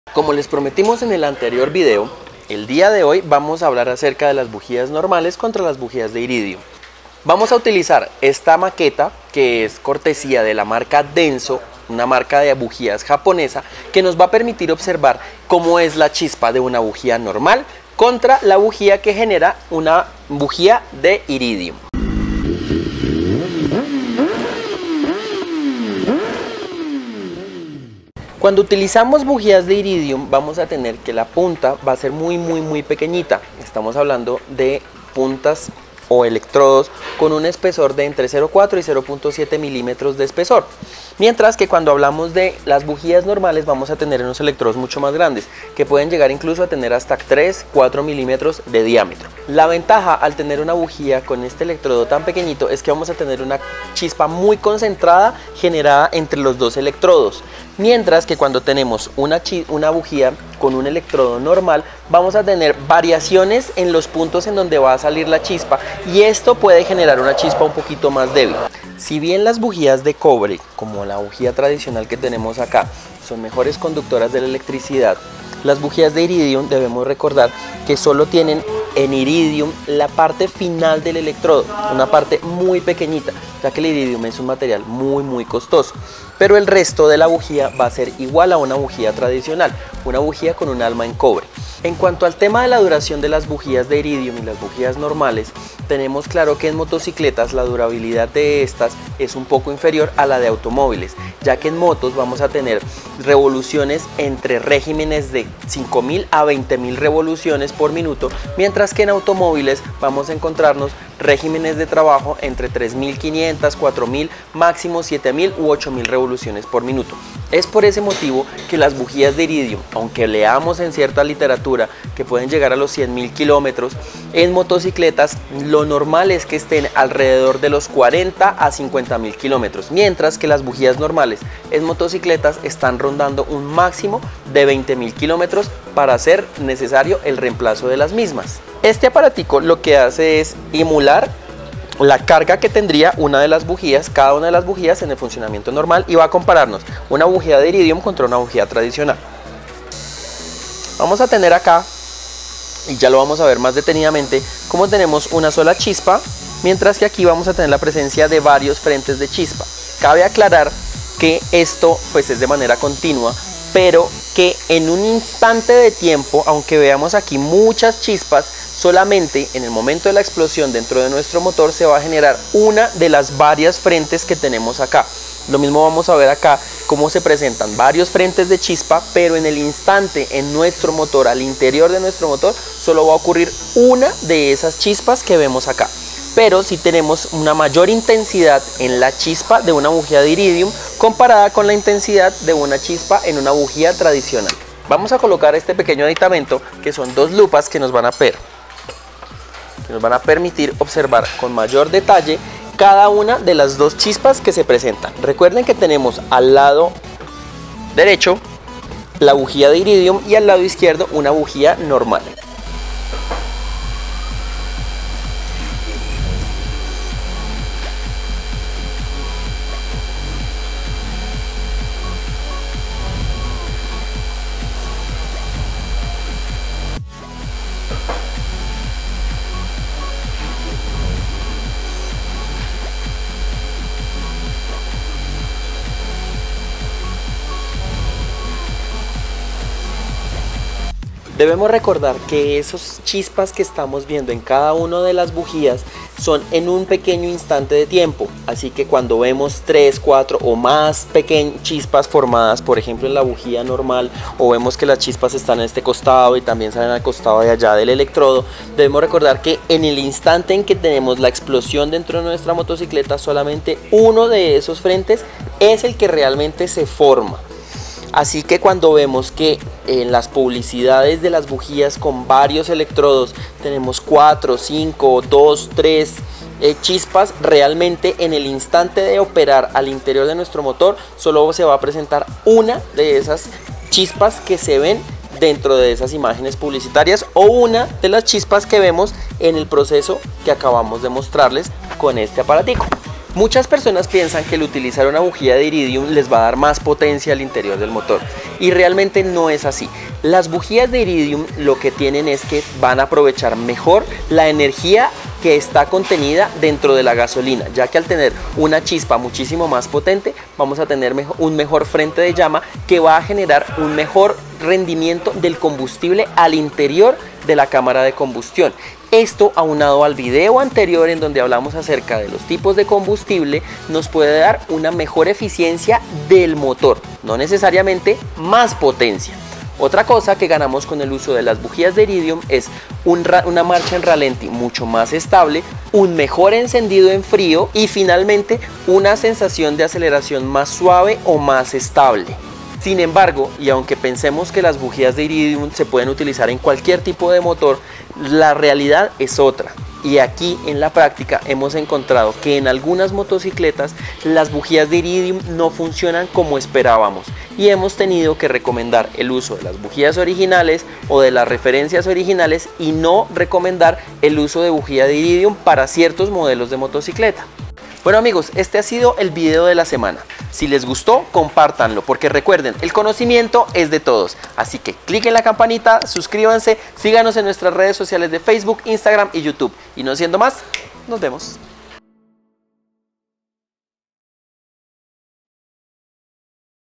bujias de iridium vs originales o de cobre
como lo prometido es deuda en el video de hace 15 días empezamos con el tema de las bujias y esta es la segunda parte que complementa el tema.

si les gusto el video compartanlo